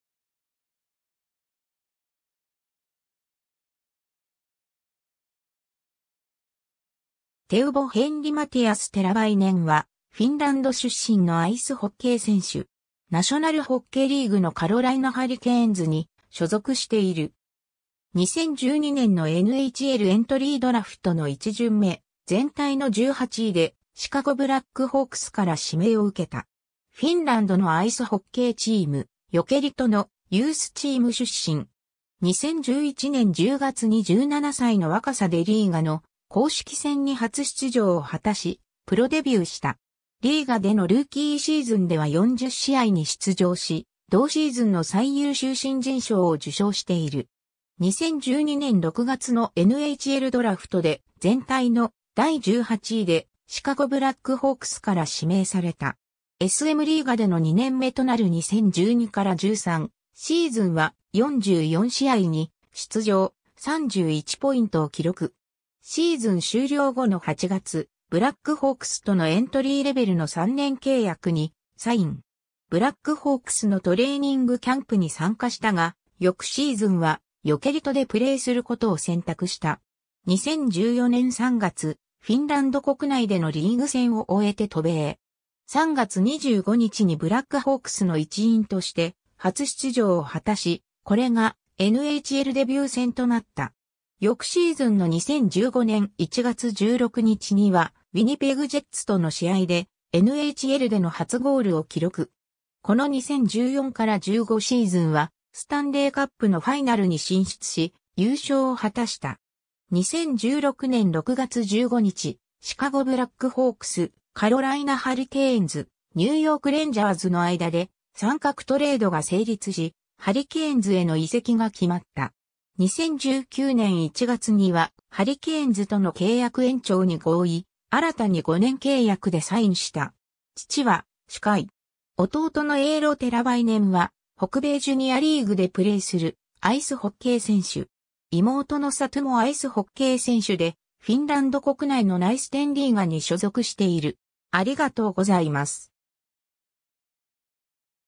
テウヴォ・テラヴァイネン
シカゴ・ブラックホークスの選手
カロライナ・ハリケーンズの選手
テウヴォ・ヘンリ・マティアス・テラヴァイネン（Teuvo Henri Matias Teräväinen、1994年9月11日 - ）は、フィンランド出身のアイスホッケー選手。
ナショナルホッケーリーグ(NHL)のカロライナ・ハリケーンズに所属している。
2012年のNHLエントリー・ドラフトの1巡目、全体の18位でシカゴ・ブラックホークスから指名を受けた。
 フィンランドのアイスホッケーチーム、ヨケリトのユースチーム出身。
2011年10月に17歳の若さでLiiga（当時の名称はSMリーガ）の公式戦に初出場を果たし、プロデビューした。
Liigaでのルーキーシーズンでは40試合に出場し、同シーズンの最優秀新人賞を受賞している。
2012年6月のNHLドラフトで、全体の第18位でシカゴ・ブラックホークスから指名された。
SMリーガでの2年目となる2012-13シーズンは、44試合に出場、31ポイントを記録。
シーズン終了後の8月、ブラックホークスとのエントリーレベルの3年契約にサイン。
ブラックホークスのトレーニングキャンプに参加したが、翌シーズンはヨケリトでプレーすることを選択した。
 2014年3月、フィンランド国内でのリーグ戦を終えて渡米。
3月25日にブラックホークスの一員として初出場を果たし、これがNHLデビュー戦となった。
翌シーズンの2015年1月16日には、ウィニペグ・ジェッツとの試合でNHLでの初ゴールを記録。
この2014-15シーズンは、スタンレー・カップのファイナルに進出し、優勝を果たした。
 2016年6月15日、シカゴ・ブラックホークス、カロライナ・ハリケーンズ、ニューヨーク・レンジャーズの間で、三角トレードが成立し、ハリケーンズへの移籍が決まった。
 2019年1月には、ハリケーンズとの契約延長に合意、新たに5年契約（総額2700万ドル）でサインした。
 父は歯科医。
弟のエエロ・テラヴァイネンは、北米ジュニアリーグでプレーするアイスホッケー選手。
妹のサトゥもアイスホッケー選手で、フィンランド国内のナイステン・リーガ（女子トップリーグ）に所属している。